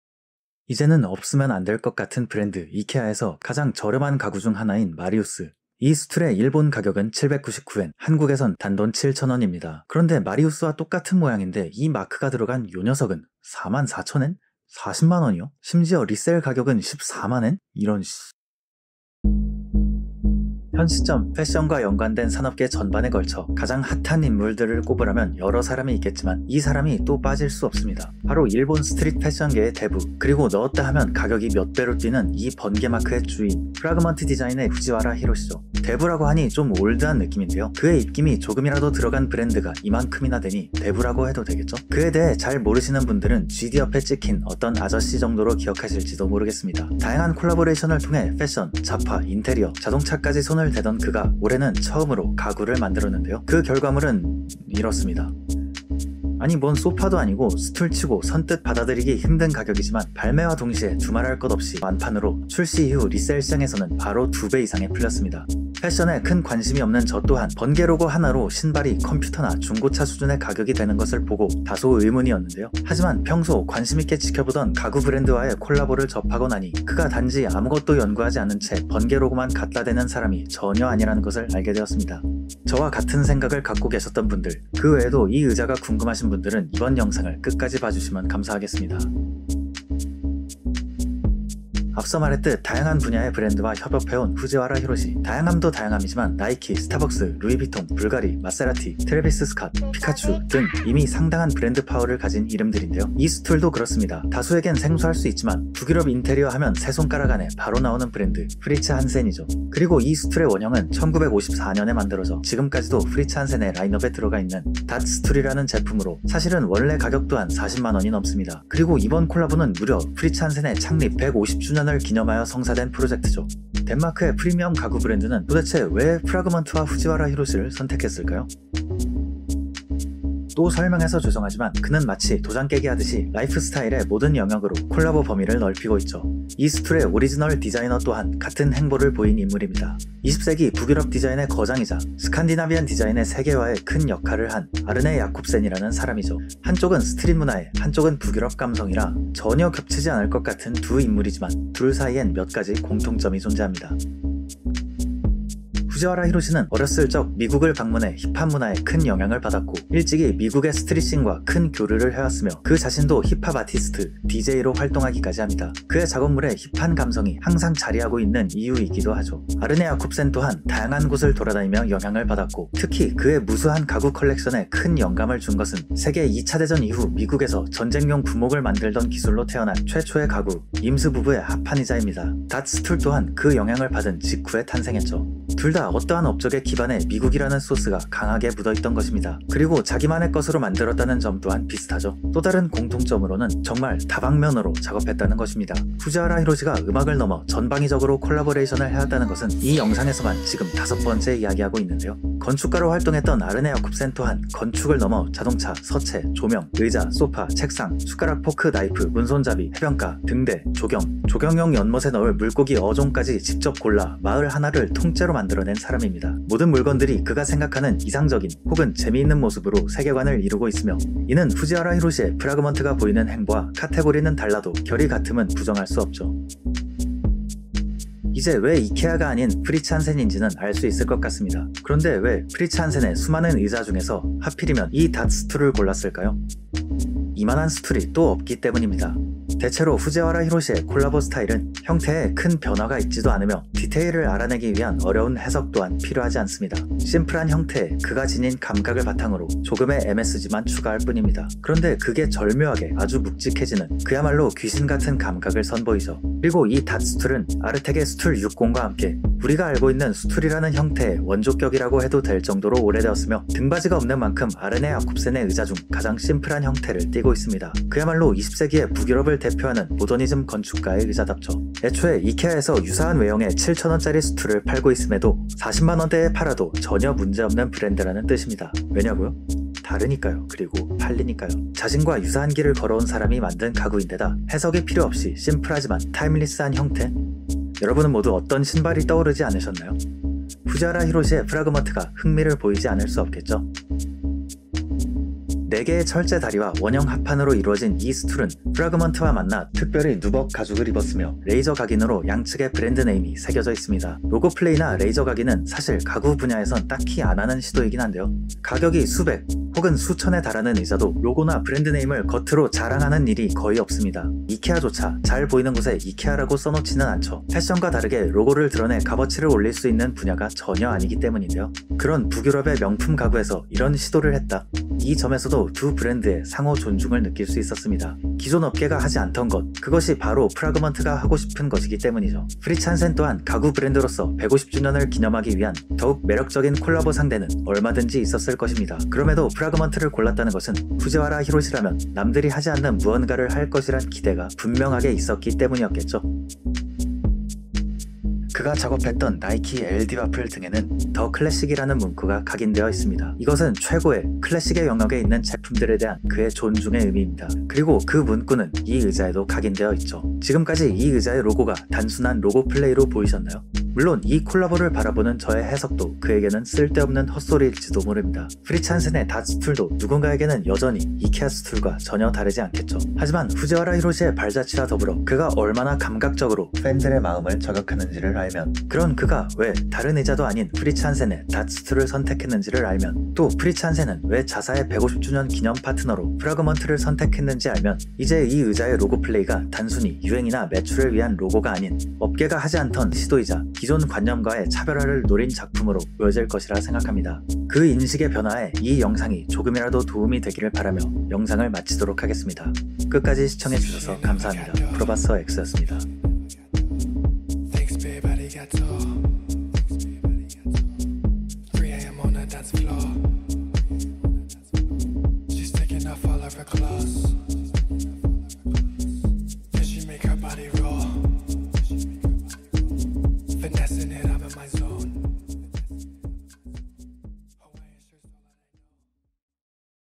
이제 가구도 손대는 프라그먼트...오히려좋아 / 프리츠한센x프라그먼트 닷 스툴 한정판 언박싱
스트릿 씬의 여포 후지와라 히로시의 프라그먼트가,
북유럽 프리미엄 가구 프리츠한센의 창사150주년을 기념하기 위해 한정판 스툴을 만들었습니다.
그의 커리어 사상 첫 가구회사와의 콜라보에서, 그는 어떤 매직을 보여줬을까요?

*해당 영상은 프라그먼트, 혹은 프리츠 한센의 공식 발표내용이 아닌, 가구에 대한 흥미와 팬심으로 제작된 주관적인 영상입니다

00:00 Intro
00:19 프라그먼트의 사상 첫 가구콜라보
01:31 이 의자의 정체는..?
02:08 이 둘은 어쩌다 만났을까..?
02:34 유사한 길을 걸어온 두 인물
03:47 그는 왜 이 의자를 골랐을까..?
04:53 프라그먼트 스툴 언박싱
05:46 후지와라 히로시의 리스펙트..